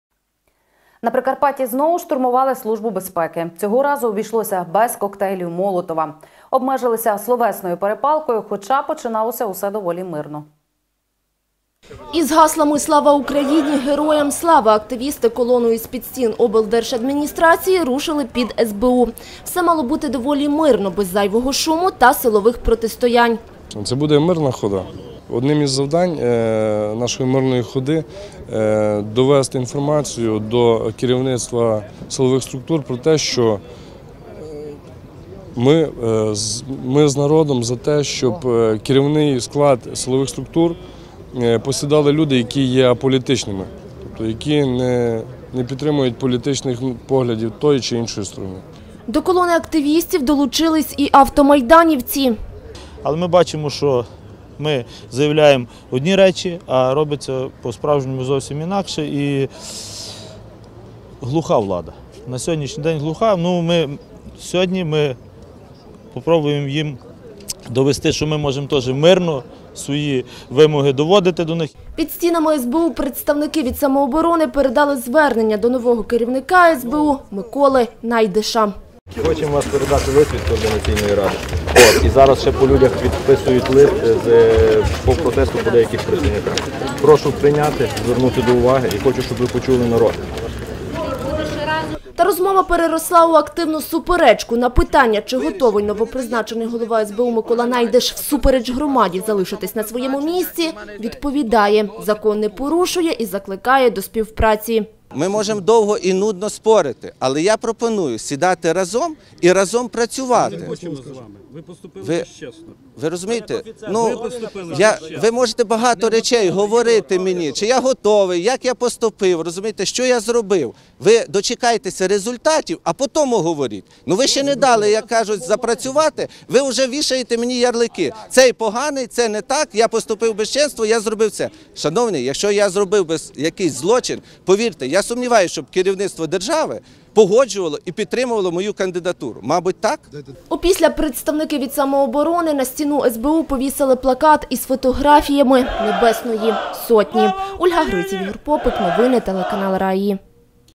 Штурм СБУ
На Прикарпатті знову штурмували Службу безпеки. Цього разу обійшлося  без «коктейлів Молотова». Обмежилися словесною перепалкою, хоча починалося усе доволі мирно.